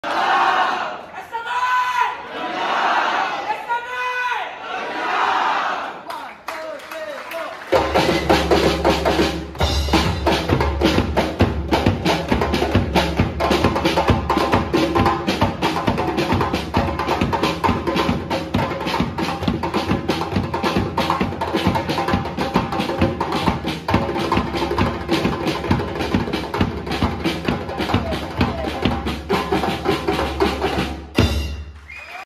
GIMIK PEMBUKAAN HARI ANUGERAH KECEMERLANGAN KOKURIKULUM SMK ST MICHAEL
Gimik Pembukaan berasaskan percussion assemble oelh Michaelian Kompang